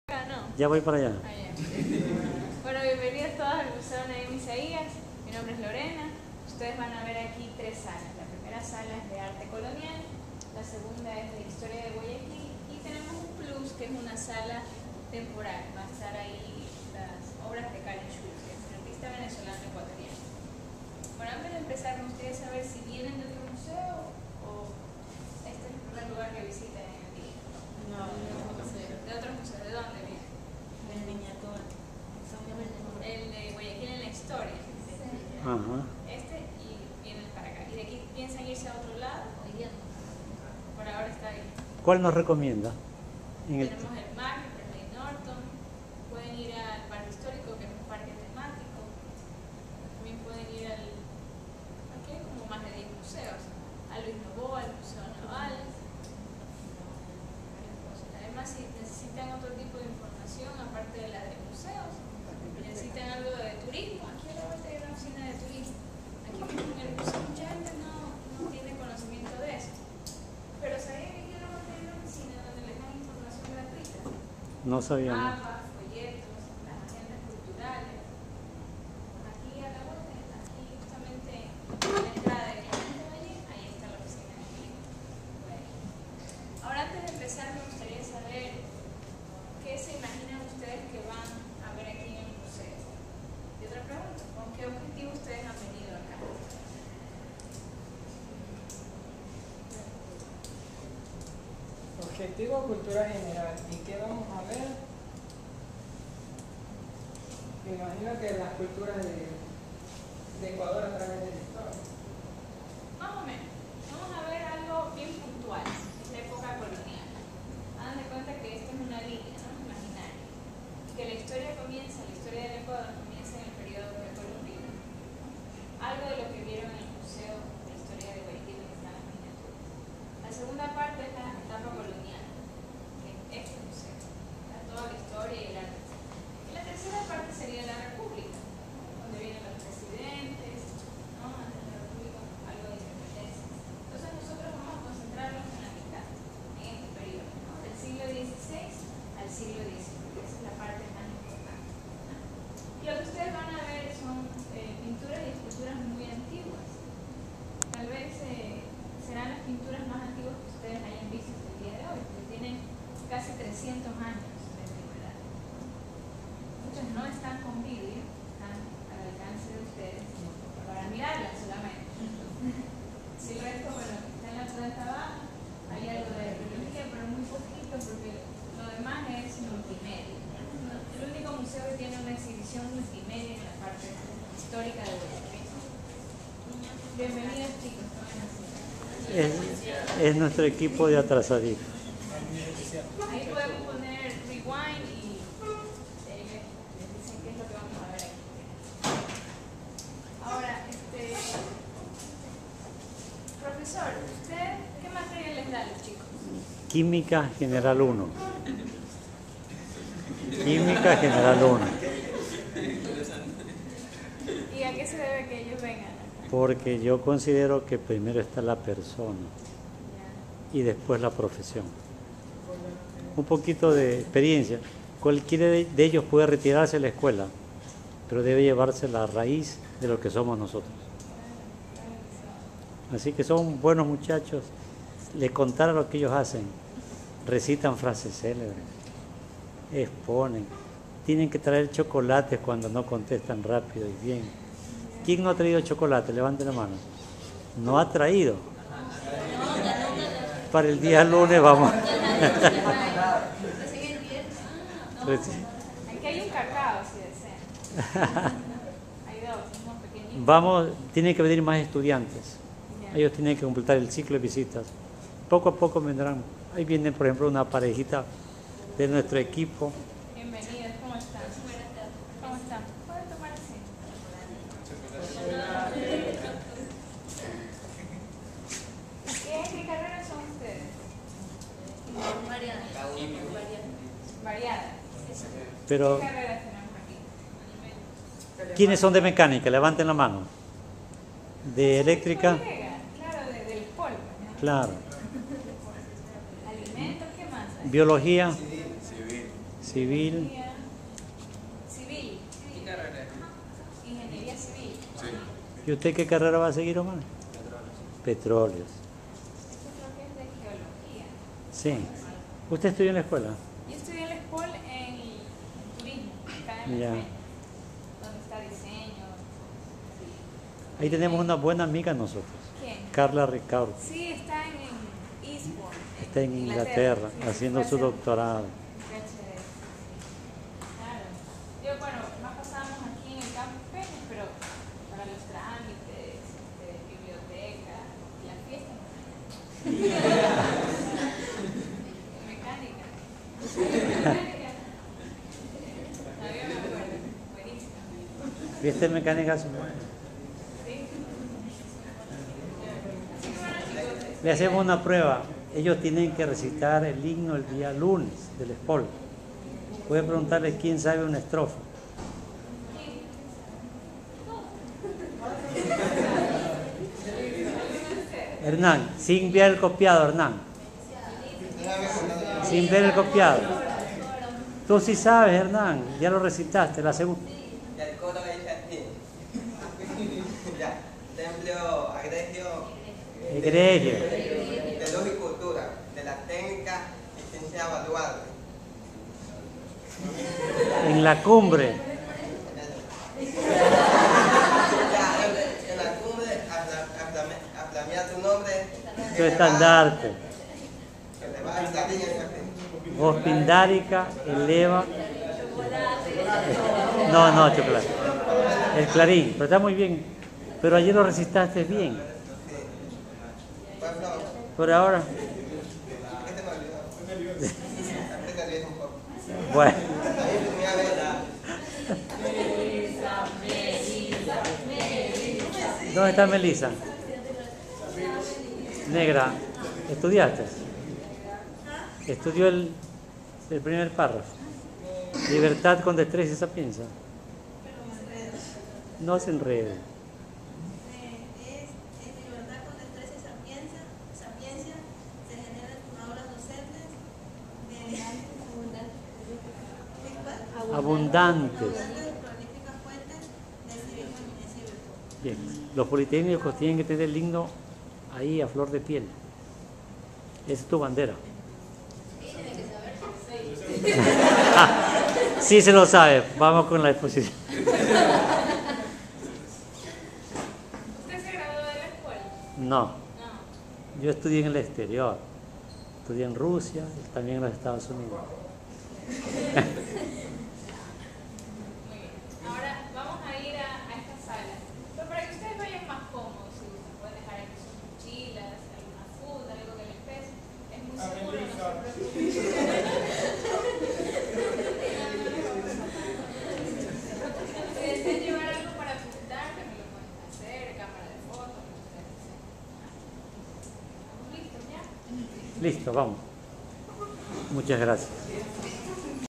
VID00561 ESPOL ICQA visita al museo Nahim Isaías de Guayaquil por novatos ESPOL 2010.01.23
El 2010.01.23 se llevo a cabo la visita al  museo Nahim Isaías de Guayaquil por parte de los estudiantes de Química General I de la ESPOL. Excelente visión didáctica e interactiva para apreciar un enorme catálogo de pinturas, esculturas en madera y piedra y otros objetos que abarcan desde el siglo XVI hasta el XIX, prestando una especial atención al arte religioso. Los alumnos novatos se llevaron algunas ideas para ser plasmadas en  el concurso semestral de emprendimiento, ciencia y tecnología del ICQA donde los proyectos se los encuadra en el lema: Primero la persona, el emprendedor, luego la profesión